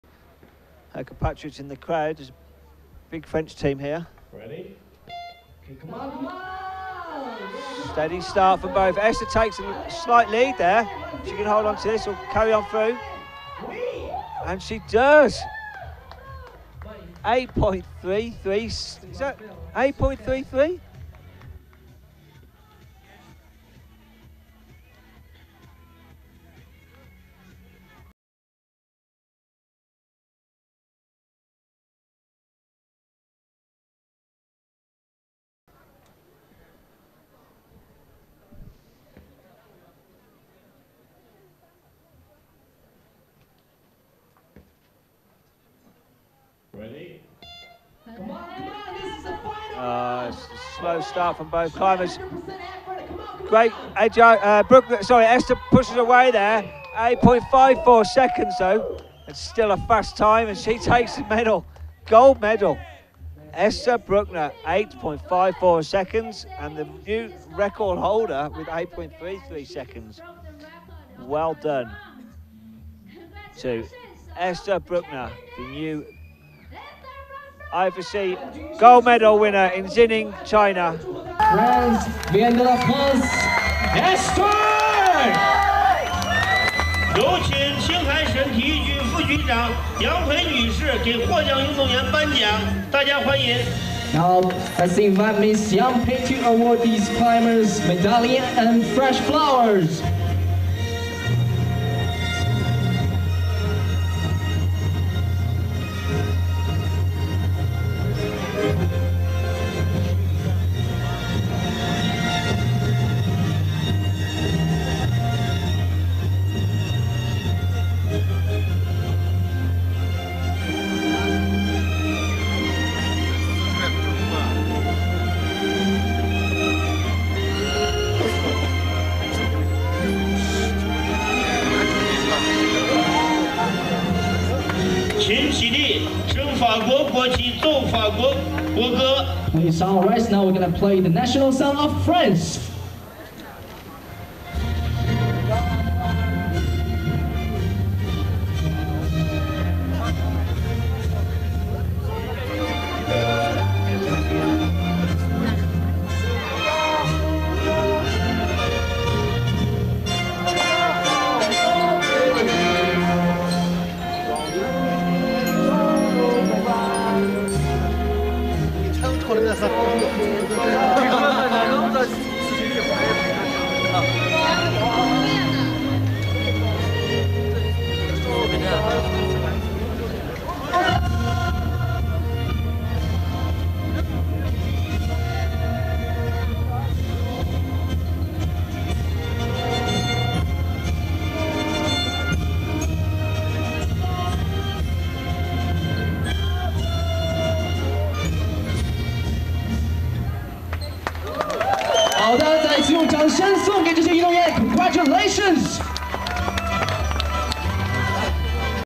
Escalade - XINING - Esther Bruckner
Médaille d'or et nouveau record du Monde pour Esther Bruckner sur cette étape de coupe du Monde